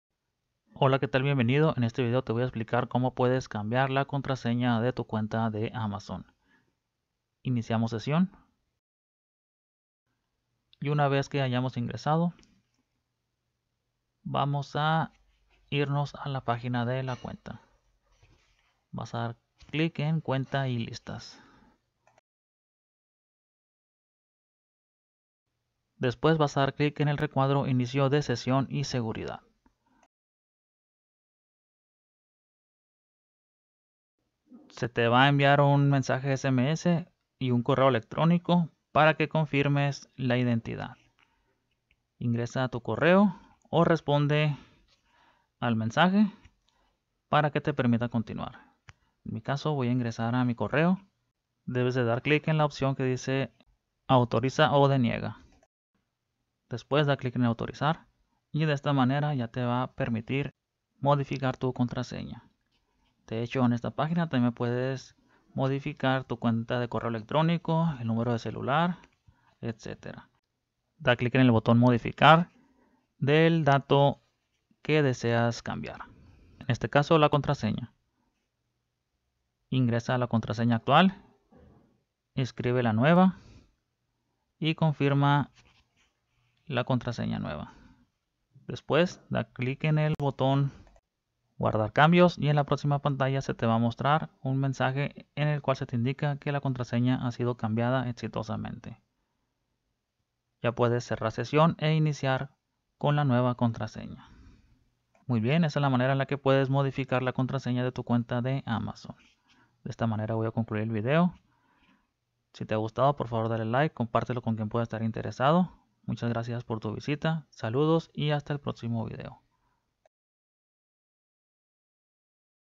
¿Cómo Cambiar Contraseña De Cuenta Amazon? Modificar Password Correo Electrónico Número De Teléfono
Guía - tutorial donde se muestra la forma en la que puedes actualizar la contraseña de tu cuenta de Amazon.

Aprende a cambiar o modificar el password, correo electrónico o número de teléfono en tu cuenta de Amazon.